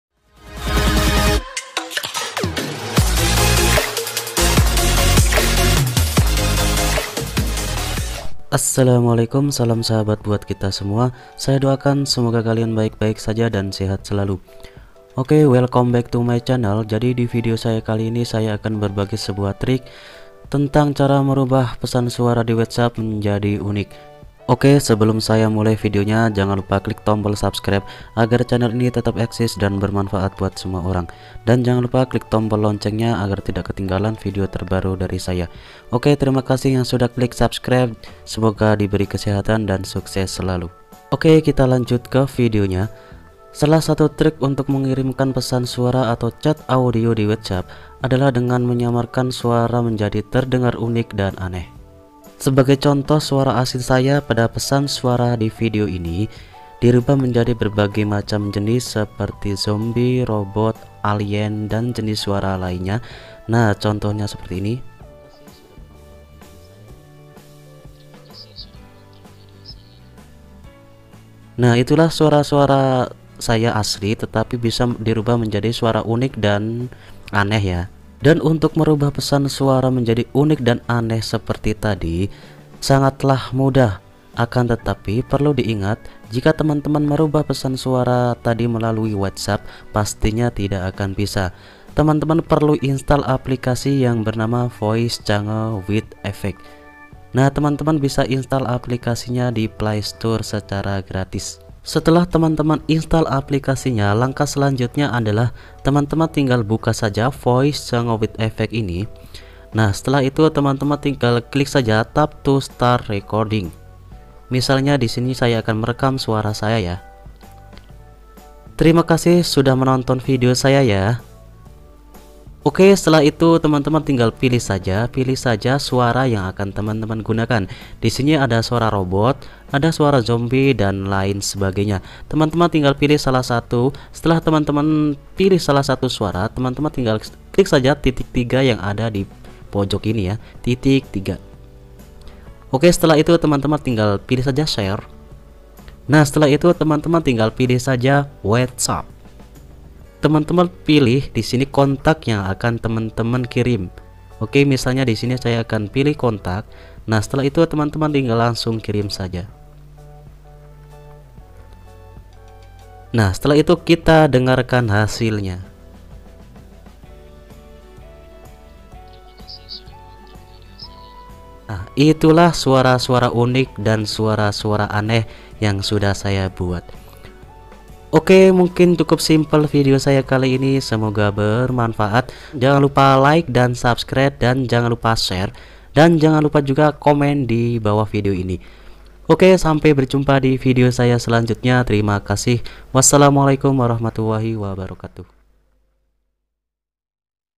Cara Merubah Pesan Suara Di Whatsapp Menjadi Unik
Salah satu trik untuk mengirimkan pesan suara atau chat audio di whatsapp adalah dengan menyamarkan suara menjadi terdengar aneh dan unik.

Sebagai contoh suara asli saya Pada pesan suara di video ini dirubah menjadi berbagai macam jenis seperti robot,alien,zombi dan jenis suara lainnya.

Untuk merubah suara asli kita menjadi aneh dan unik seperti tadi ternyata sangat mudah akan tetapi apabila merubah langsung melalui whatsapp tidak akan bisa namun dengan sebuah aplikasi silahkan download aplikasinya disini

Terimakasih